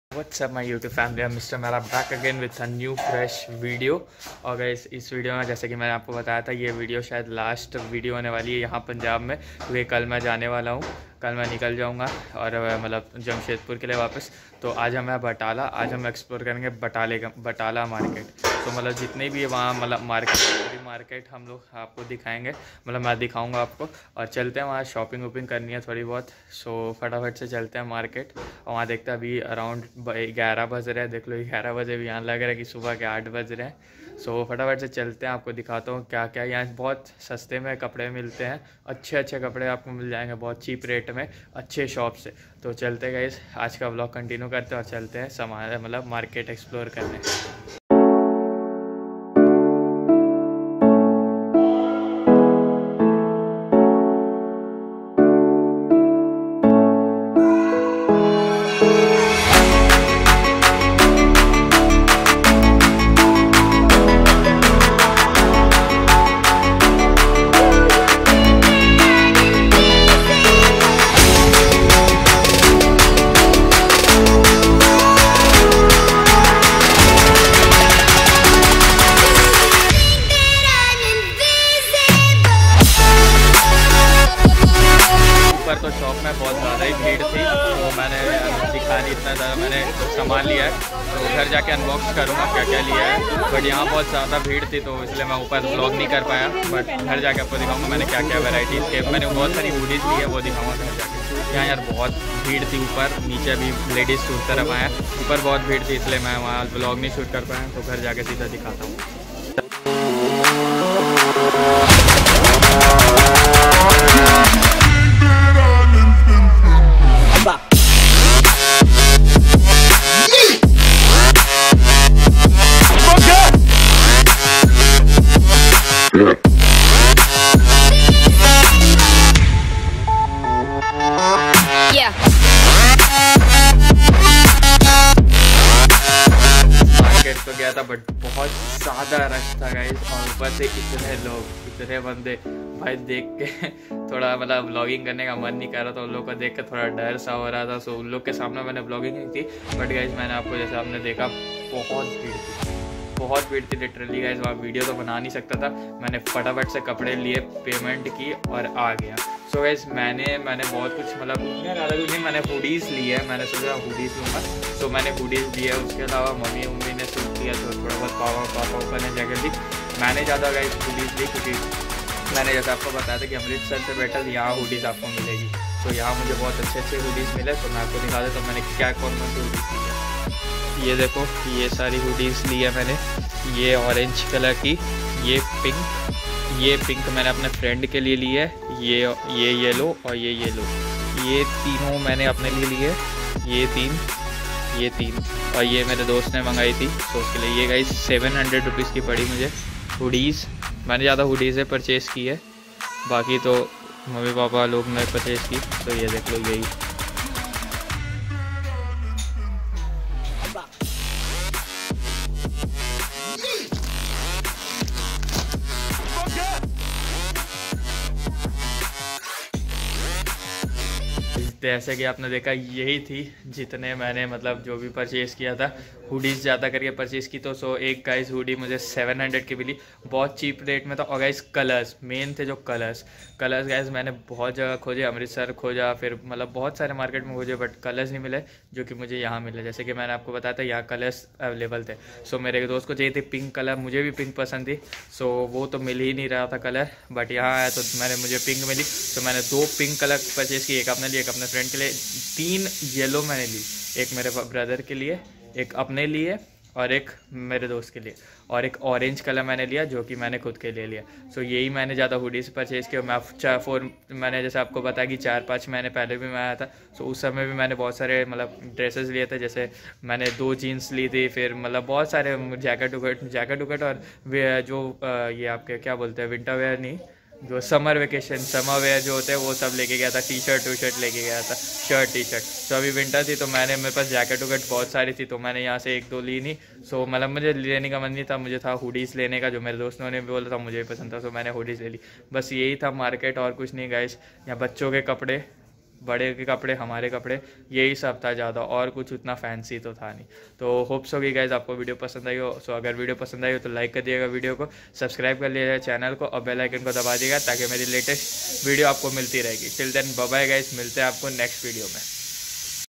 Exploring City Batala, Punjab😍! Hoodies Starting From Rs700😱😍
Thankyou guys for watching….
Gurprit Singh.